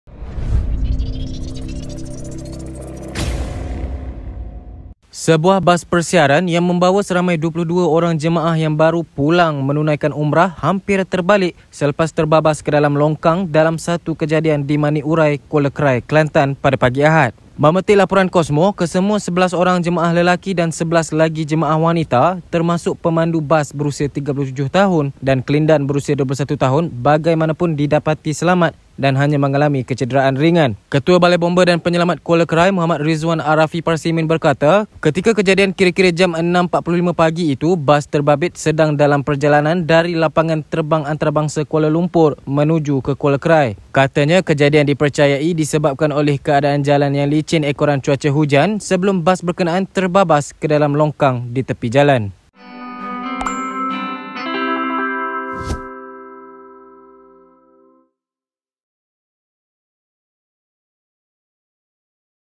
Bas bawa jemaah umrah pulang ke Kelantan terbabas
SEBUAH bas persiaran yang membawa seramai 22 orang jemaah yang baru pulang menunaikan umrah hampir terbalik selepas terbabas ke dalam longkang dalam satu kejadian di Manek Urai, Kuala Krai, Kelantan pada pagi Ahad.

- 27 Februari 2022